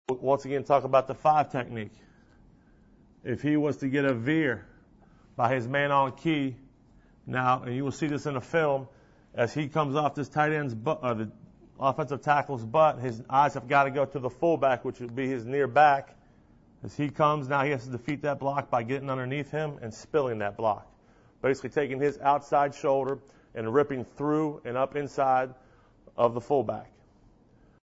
Defensive Line and Linebacker Play in the 4-4 Defense
Coaches Gillenwater and Price demonstrate the basics for the defense that helped lead the James Madison Dukes to the 2004 NCAA Division I-AA National Championship.